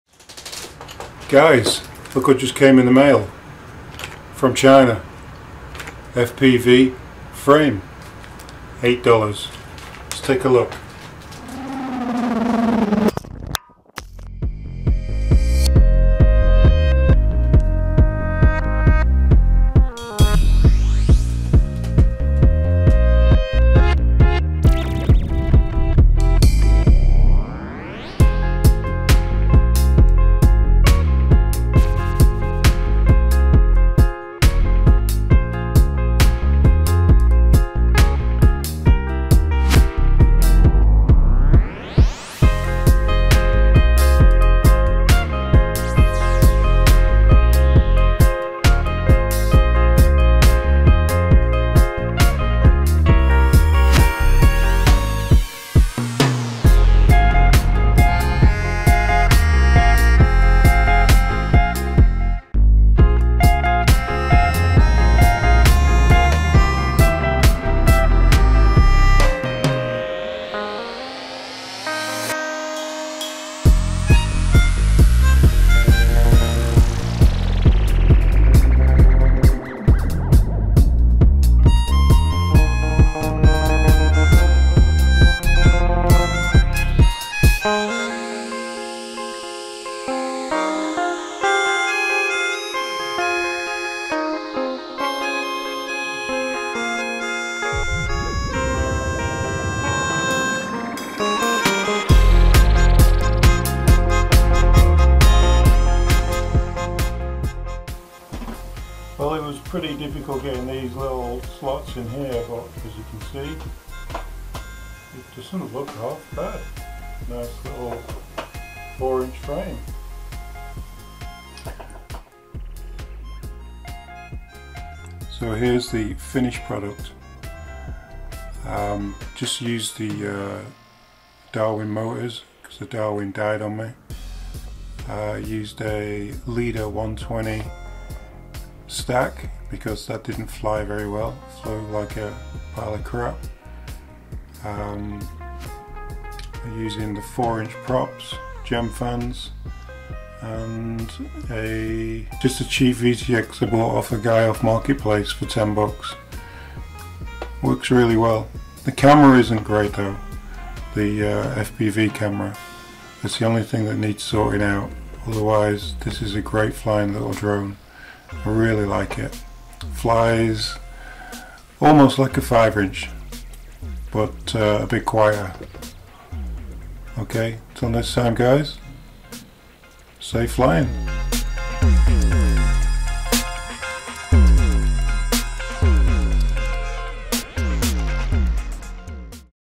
FPV 250g Freestyle frame worth buying?
We buy the $8 US currency frame from Aliexpress and build the cheapest quad ever, all in price maybe under $35 Canadian using bits we have in the parts bin